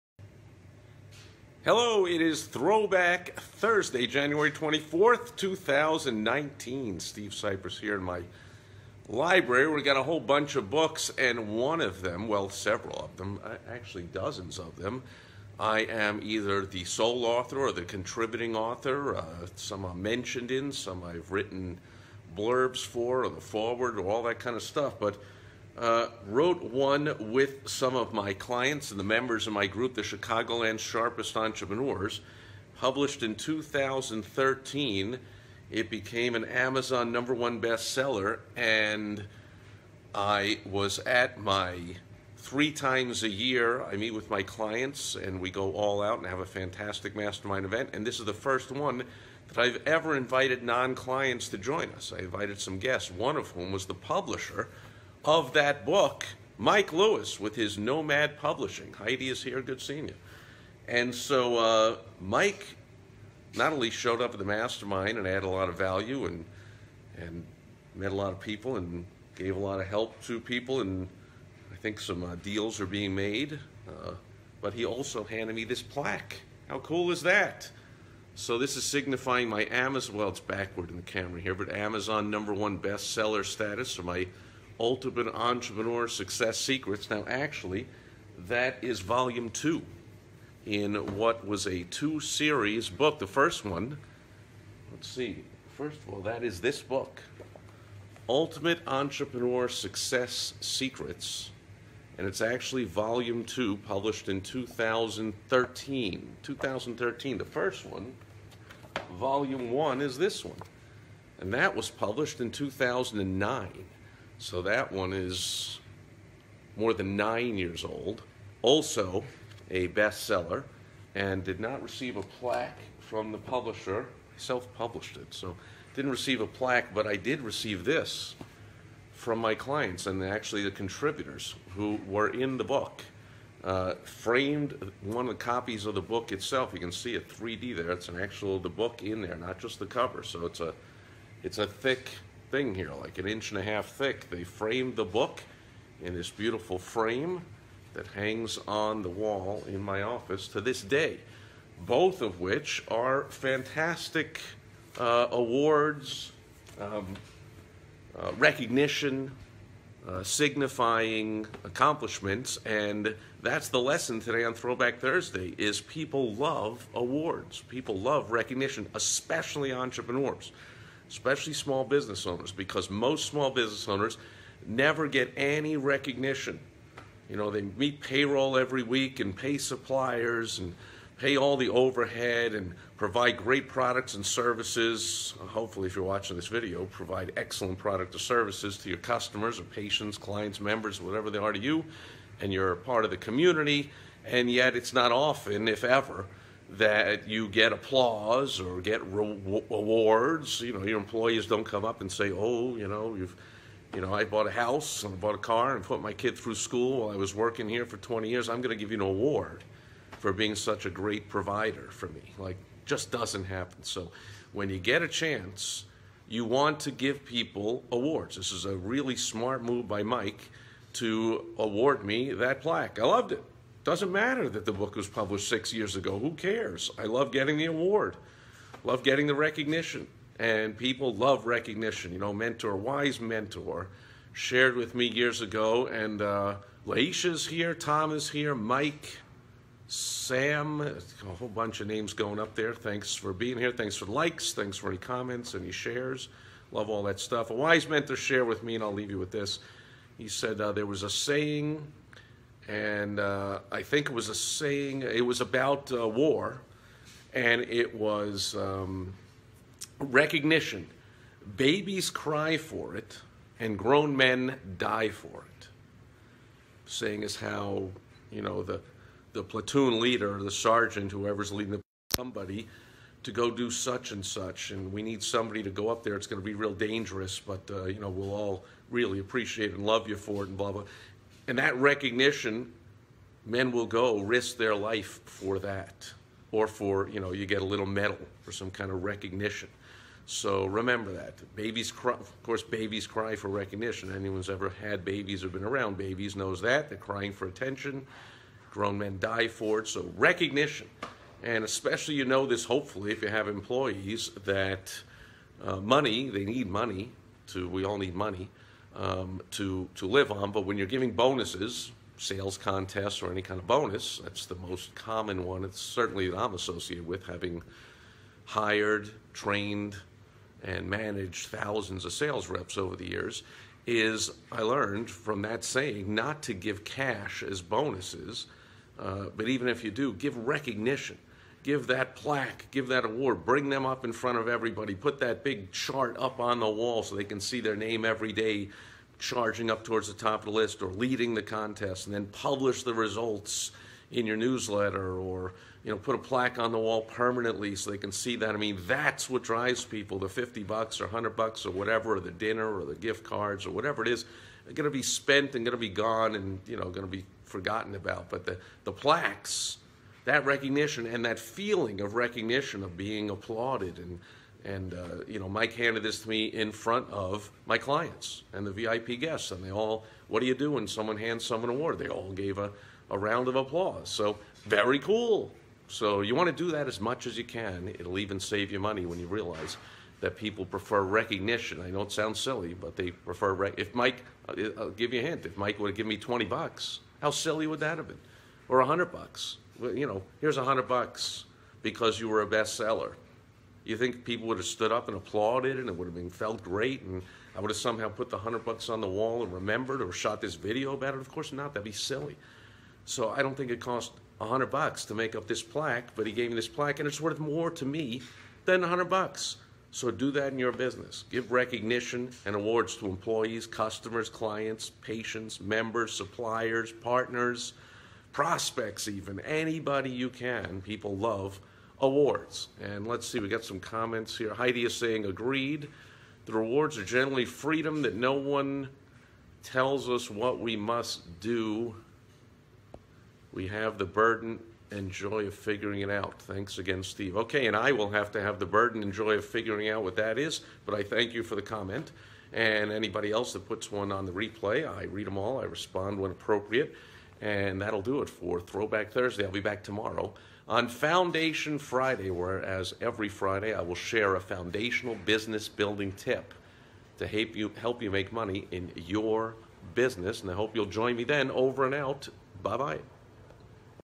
Throwback Thursday - My Best-Seller Award
Published about 6 years ago, just received a plaque for it last week.